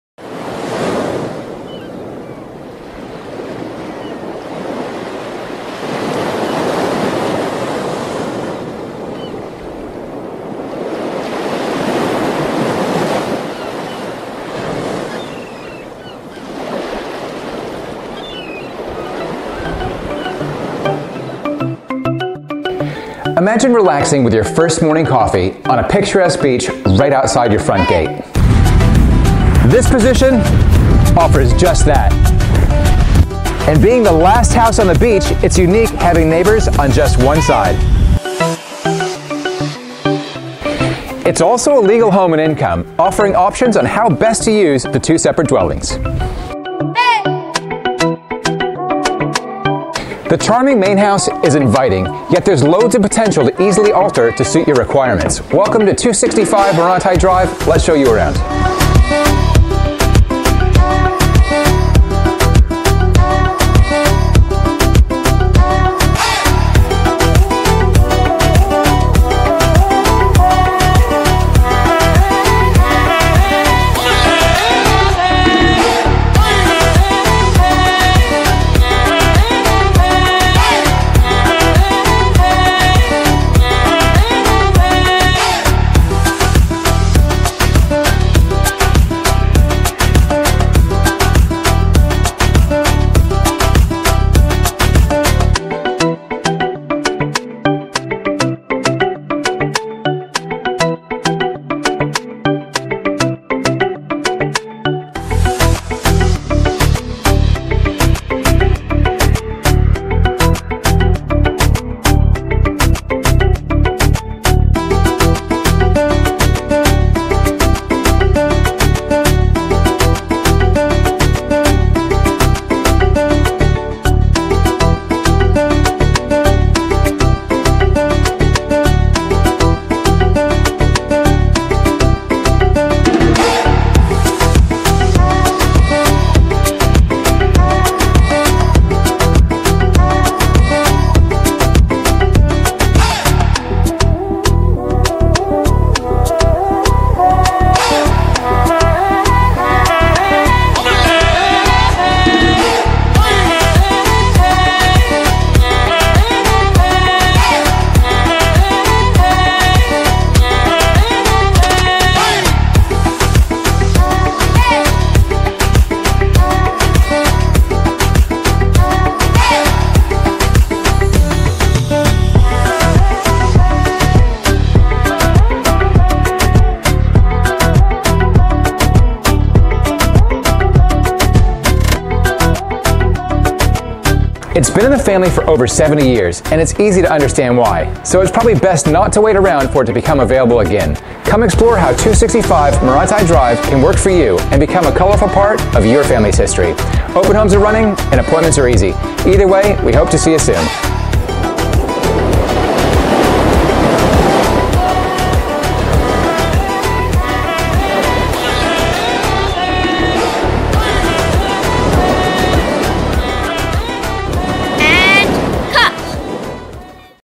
SOLD - 265 Maraetai Drive, Maraetai - Ian Boswell and Nicolette Hale
YOUR SAFE HAVEN

If you're one of our expats moving back to beautiful New Zealand, or a Kiwi who's already home and looking for a special place to settle, then this unique waterfront property could be your very ...

Property Marketed by: Ian Boswell, Beachlands and Nicolette Hale, Beachlands